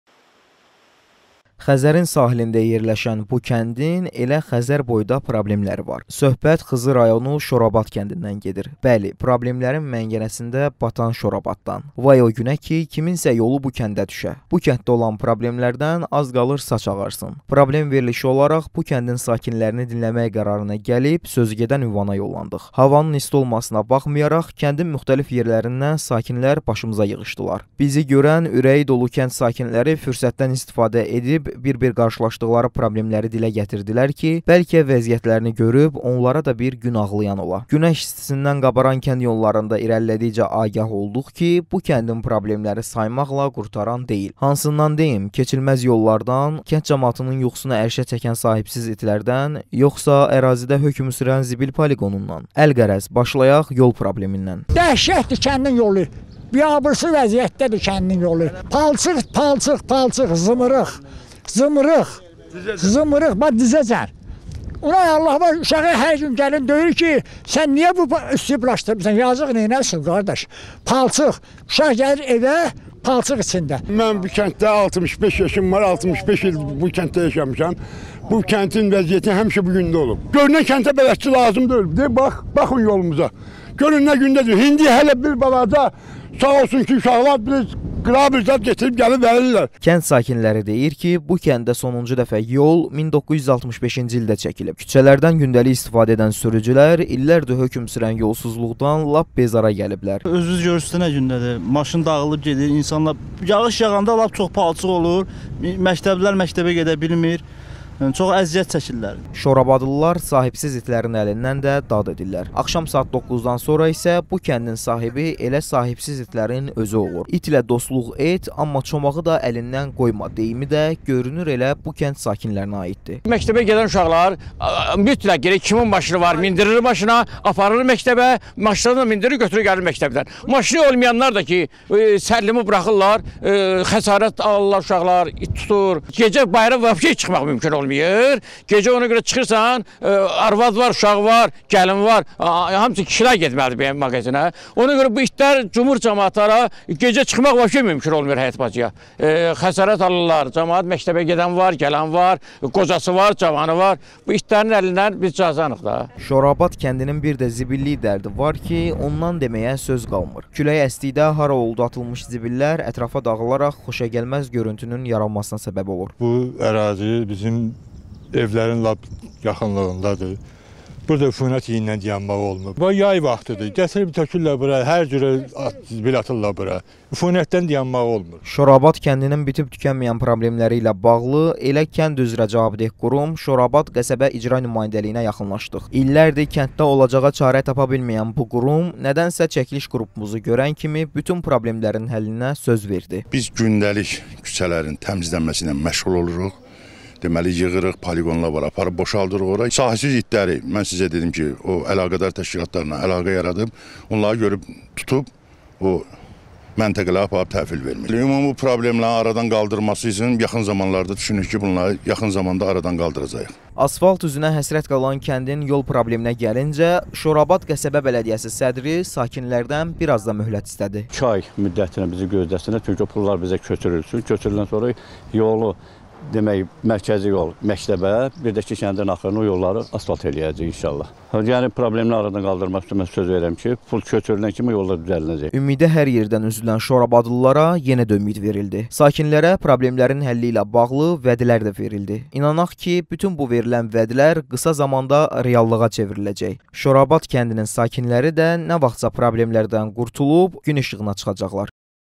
Abadlığa həsrət Şorabad qəsəbəsi
Məsul qurumlar problemlərin həlli üçün sakinlərdən möhlət istədi